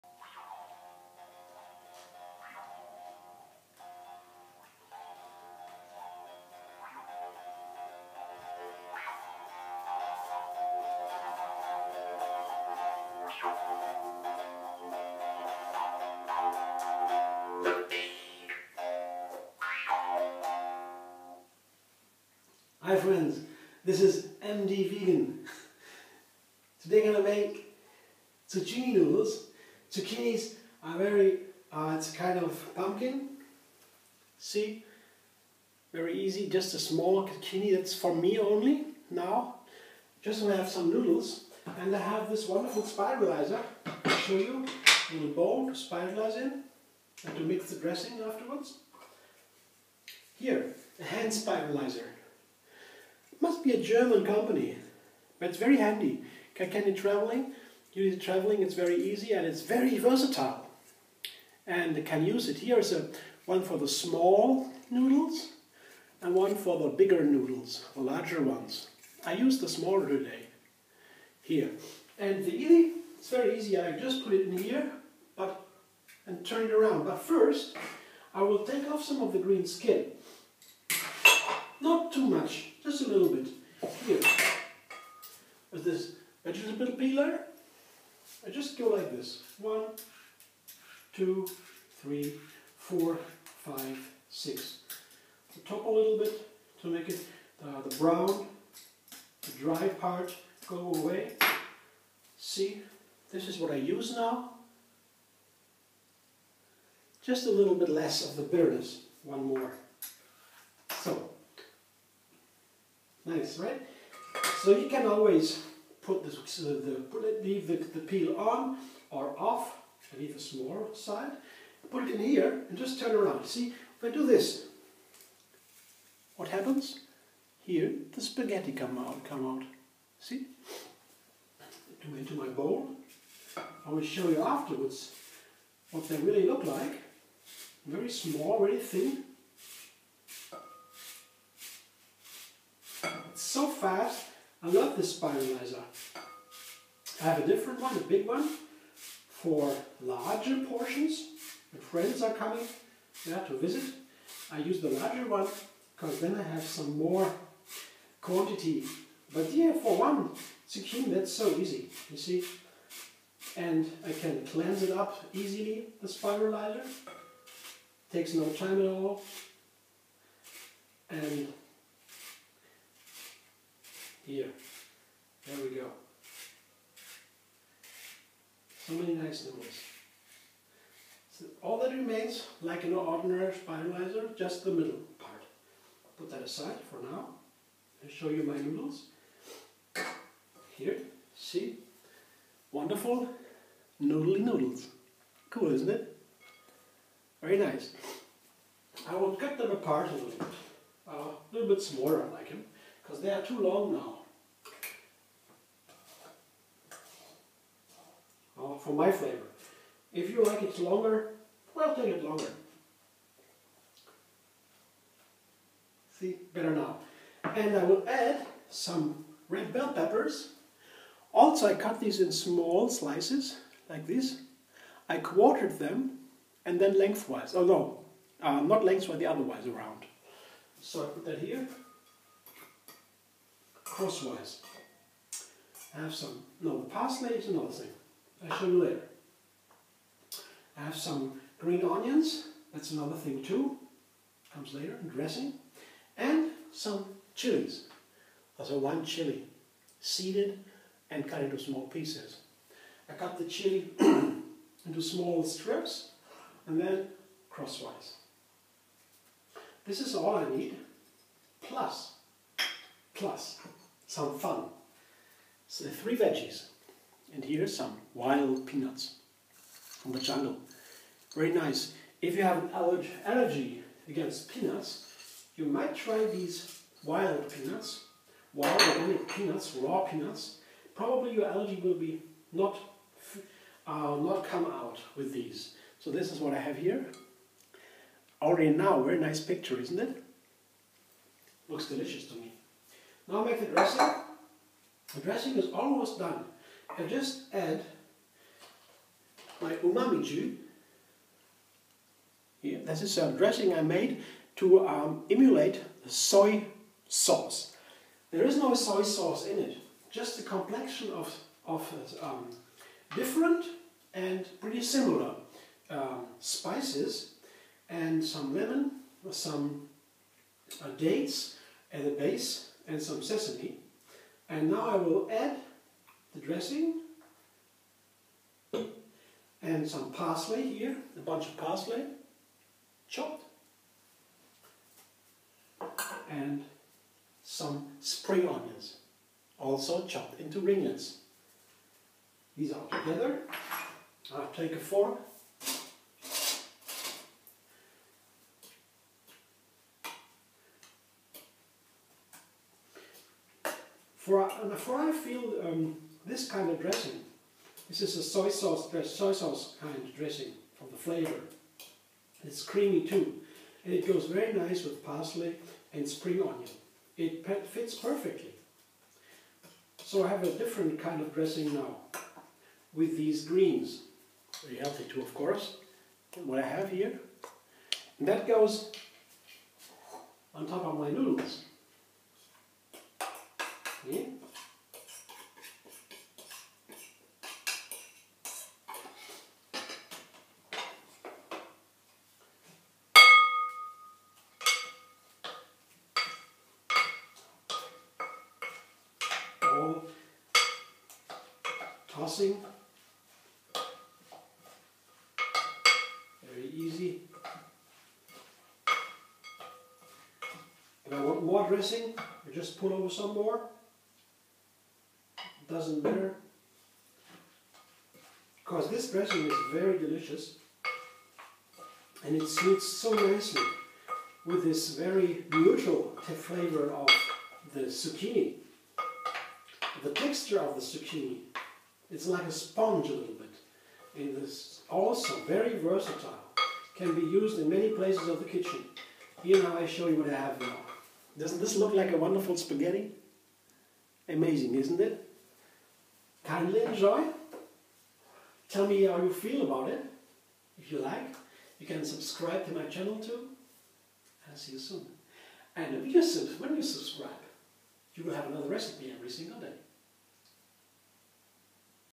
Zucchini Noodles with Asian Parsley Sauce ~ Live Entree
MD Vegan spiralizing a zucchini, combining with red bell pepper, and chili peppers, then adding chopped parsley and spring onion to Umami Jus to toss the noodles.